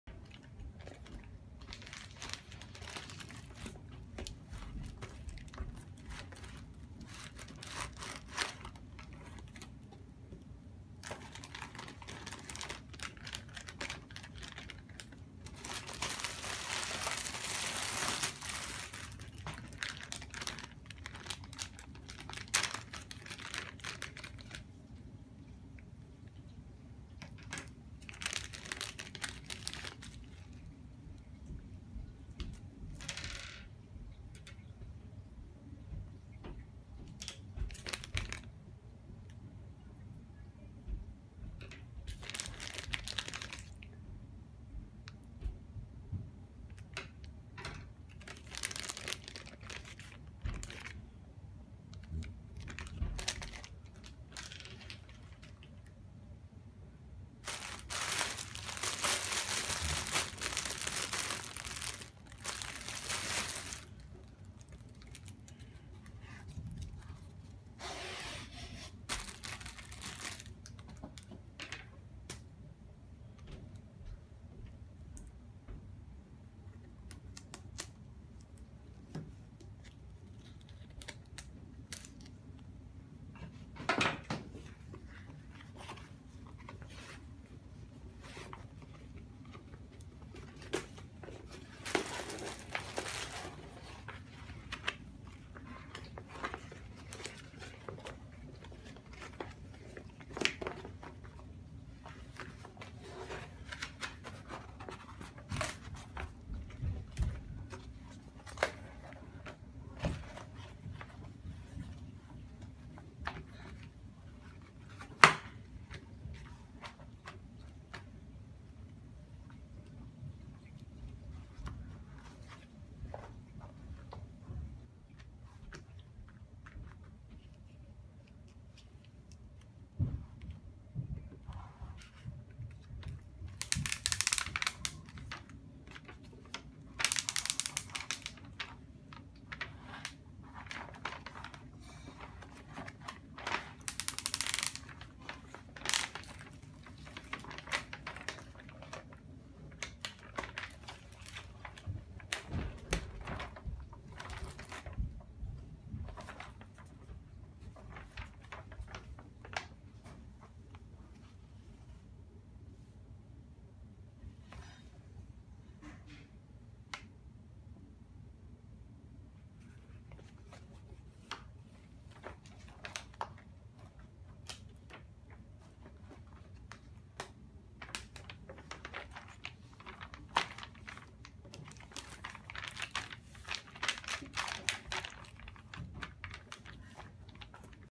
Unboxing Zooya Diamond painting with ASMR sound | Alice in the wonder land diamond painting
ZOOYA Factory Store-